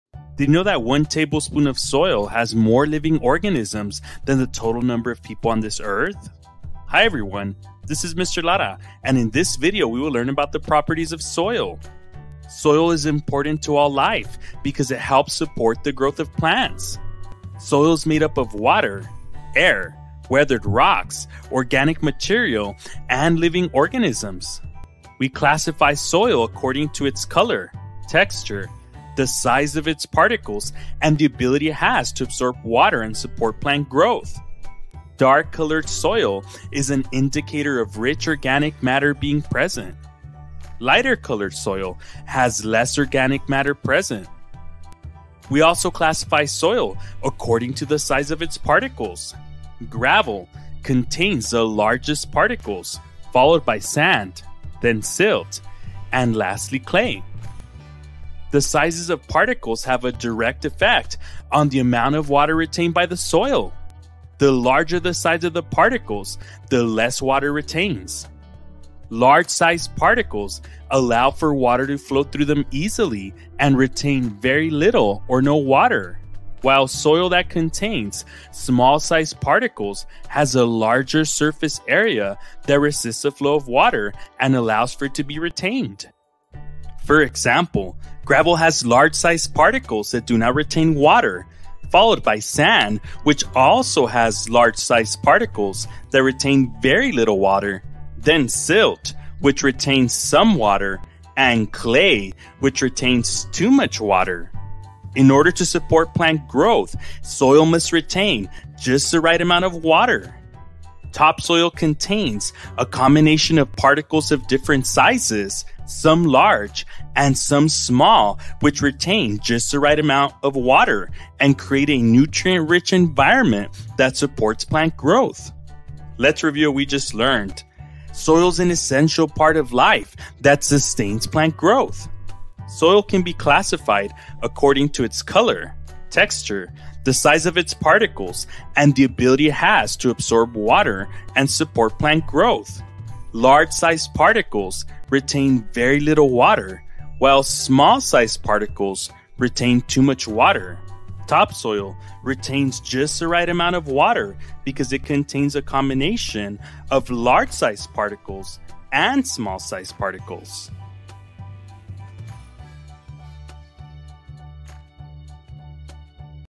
Properties of Soil [Gravel, Sand, Silt, Clay, & Topsoil]
Soil is important to all life because it helps support the growth of plants. Soil is made up of water, air, weathered rocks, organic material, and living organisms.

We classify soil according to its color, texture, the size of its particles, and the ability it has to absorb water and support plant growth.

Dark-colored soil is an indicator of rich organic matter being present.

Lighter-colored soil has less organic matter present.

We also classify soil according to the size of its particles. Gravel contains the largest particles, followed by sand, then silt, and lastly clay.

The sizes of particles have a direct effect on the amount of water retained by the soil. The larger the size of the particles, the less water that it retains.

Large-sized particles allow for water to flow through them easily and retain very little or no water while soil that contains small-sized particles has a larger surface area that resists the flow of water and allows for it to be retained.

For example, gravel has large-sized particles that do not retain water, followed by sand which also has large-sized particles that retain very little water, then silt which retains some water and clay which retains too much water.

In order to support plant growth, the soil must retain just the right amount of water.

Topsoil contains a combination of particles of different sizes, some large and some small, which retain just the right amount of water and create a nutrient-rich environment that supports plant growth.

Let's review what we just learned:

Soil is an essential part of life that sustains plant growth
soil can be classified according to its color, texture, the size of its particles, and the ability it has to absorb water and support plant growth.

Large-sized particles retain very little water, while small-sized particles retain too much water.

Topsoil retains just the right amount of water because it contains a combination of large-sized and small-sized particles.